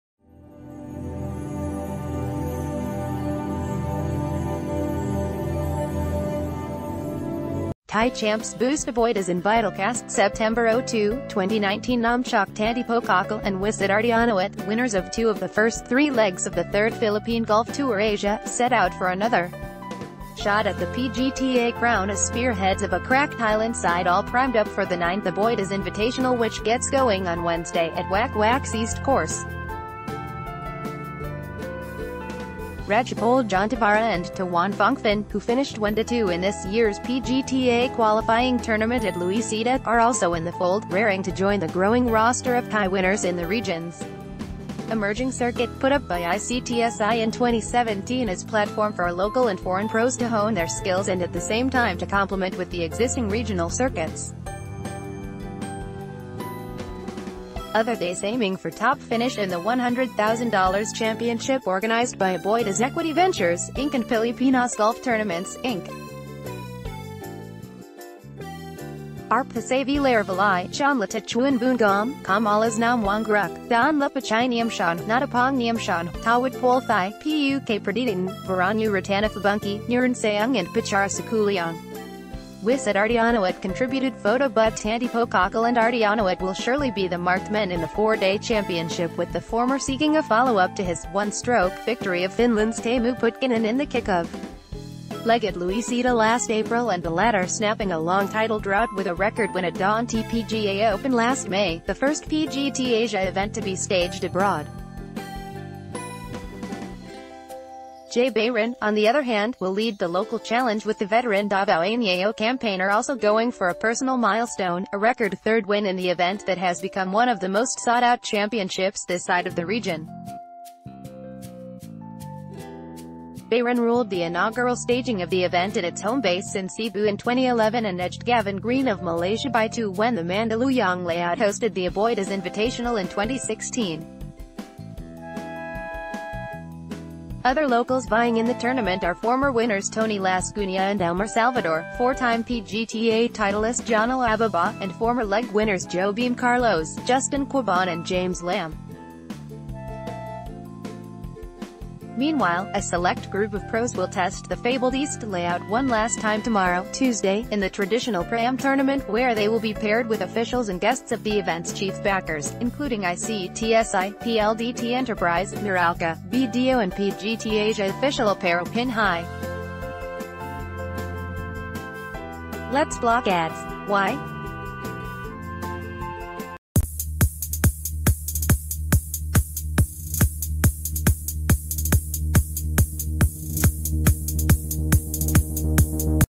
Thai champs boost Aboitiz Invit’l cast - The Manila Times
Thai champs boost Aboitiz Invit’l castSeptember 02, 2019Namchok Tantipokhakul and Wisut Artjanawat, winners of two of the first three legs of the third Philippine Golf Tour Asia, set out for another shot at the PGTA crown as spearheads of a crack Thailand side all primed up for the ninth Aboitiz Inv...